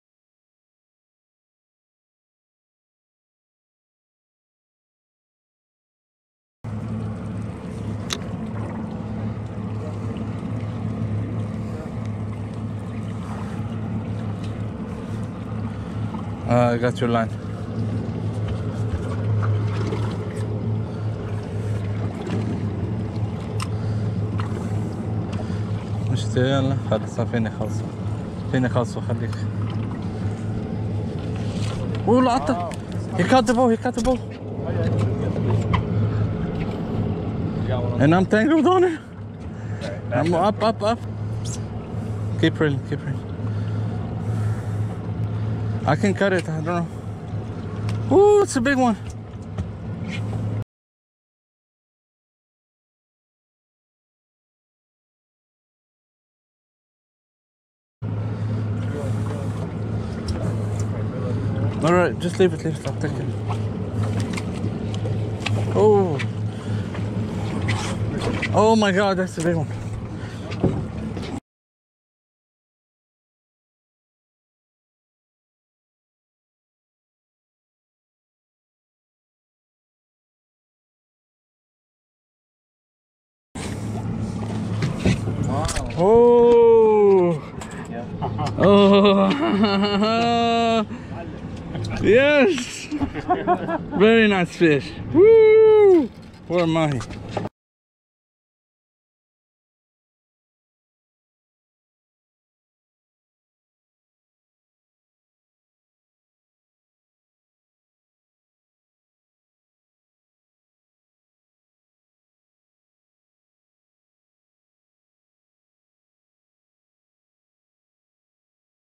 سمكة غريبة الشكل ورائعة الألوان مع شرح طريقة الصيد ماهي ماهي اللمبوكة دورادو حرباية
سمكة غريبة الشكل ورائعة الألوان مع شرح طريقة الصيد ماهي ماهي اللمبوكة دورادو